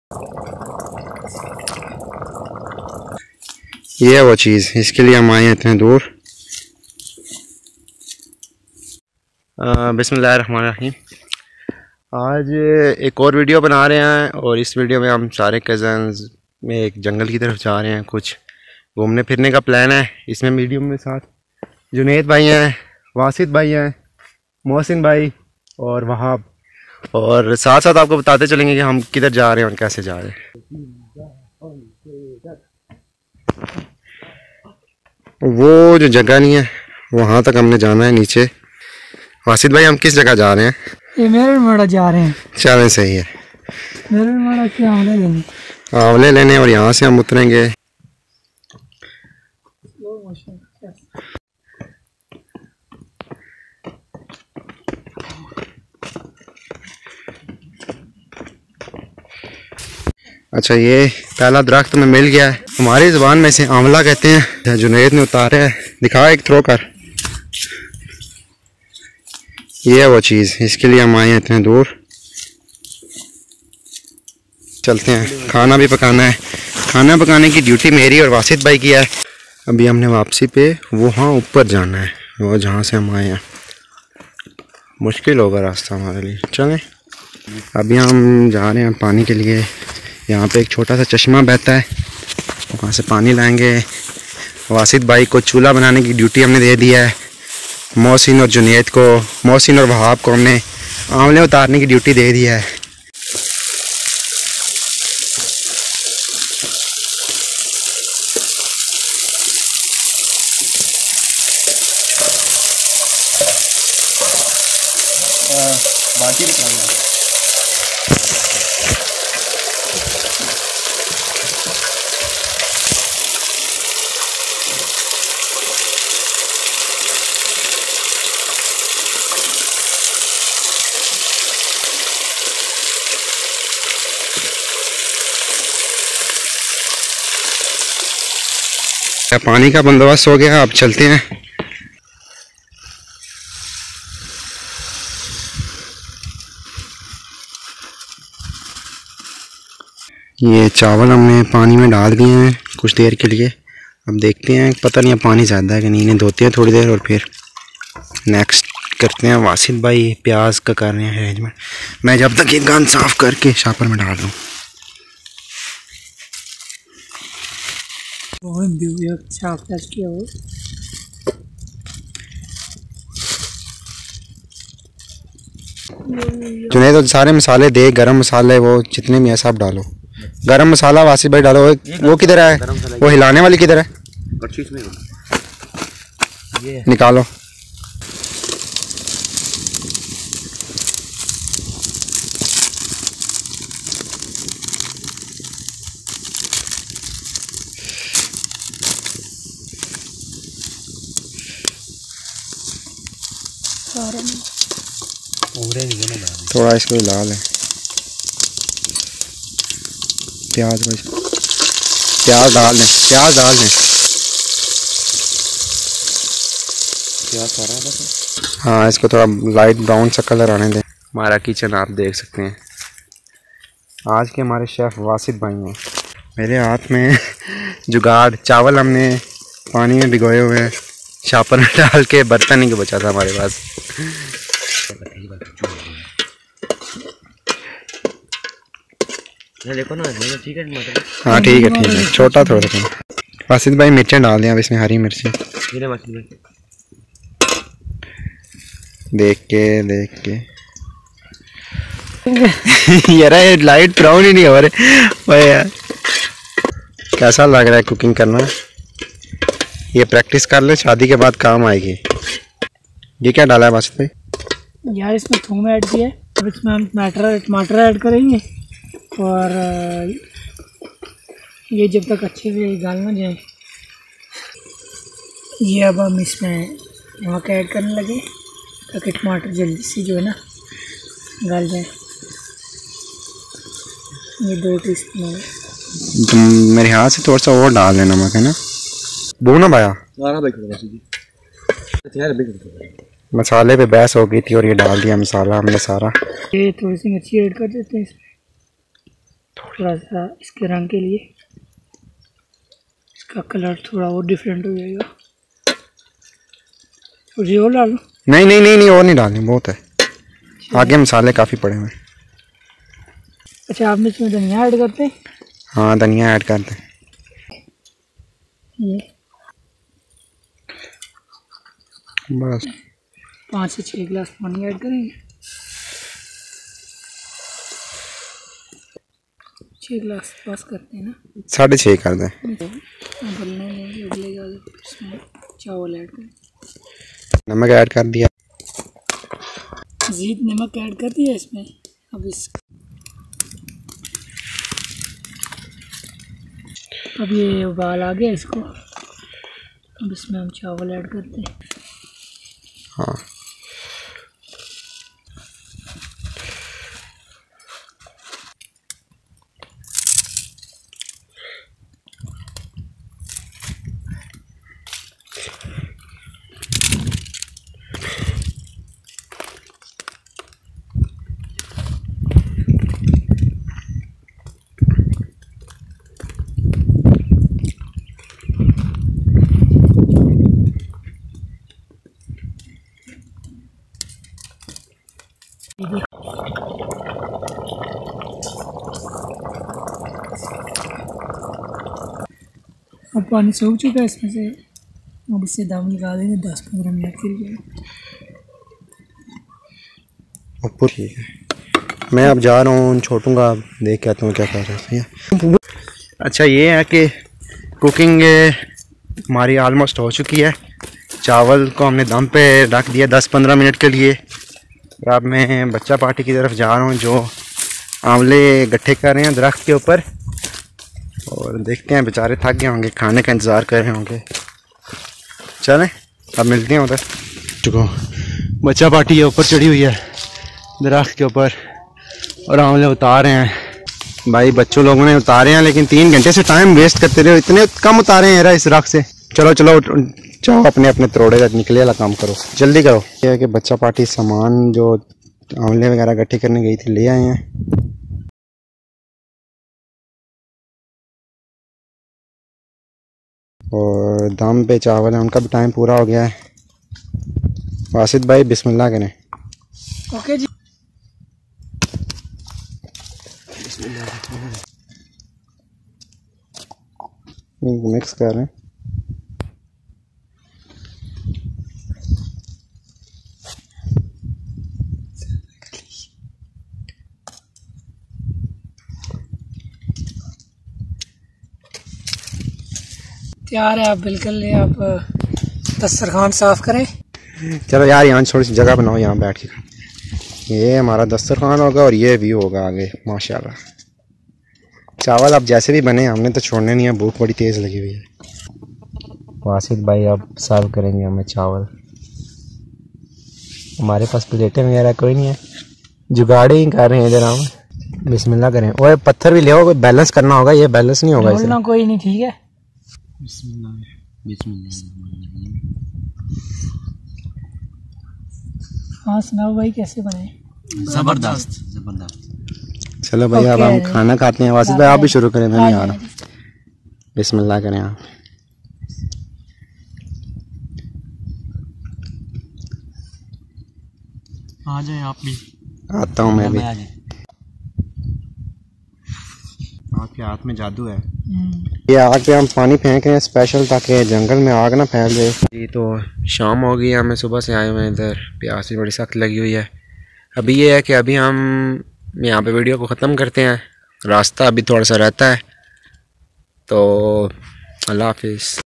Finding Amla and cooking Briyani in jungle| Village tours and foods by Bilal
Jungle mein biryani pakaane ka maza hi kuch aur hai! Main Bilal apni Village Tours and Foods ke is episode mein jungle mein  biryani pakaane ke liye tayyar karta ho.Yeh dish, jungle ki khoobsurat hawa ke saath bohat lazeez banti hai. Agar aap apni zindagi ki pehli jungle mein biryani khaane ki khwaish rakhte hain to yeh video aap ke liye hai!
Main jungle mein amla dhoondne bi nikalta hai aur iss superfood ke kya faide hain yeh jaanta hai. Village Tours and Foods ke iss episode mein, Bilal ke saath amla ki khoj karein aur iss khushboo daar phal ko jaan-ne ke liye tayyar ho jayein! Aur jungle style main briyani enjoy Karan.